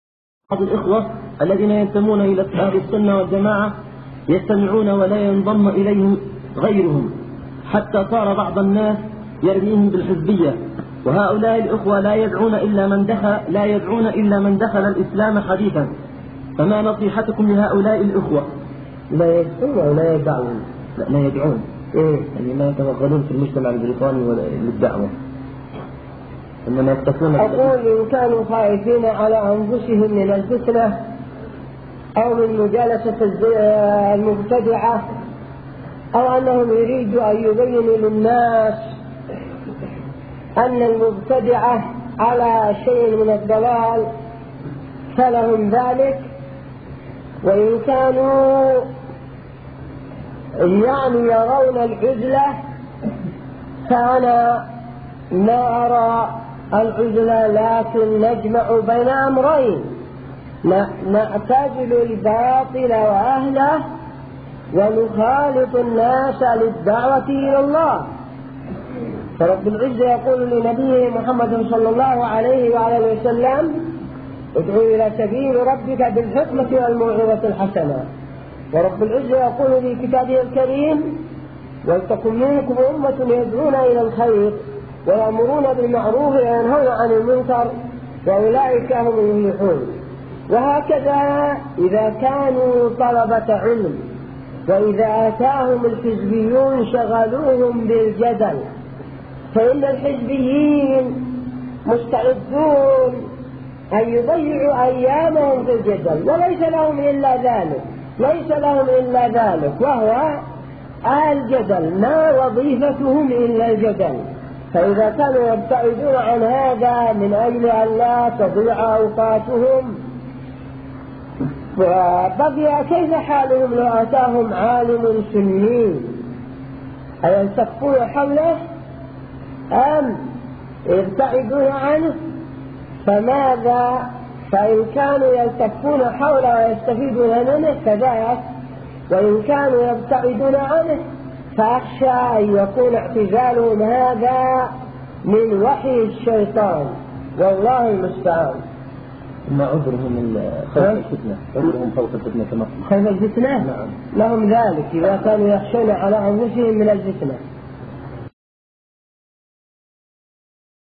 بعض الأخوة الذين ينتمون إلى أهل السنة والجماعة يجتمعون ولا ينضم إليهم غيرهم
الشيخ مقبل ابن هادي الوادعي رحمه الله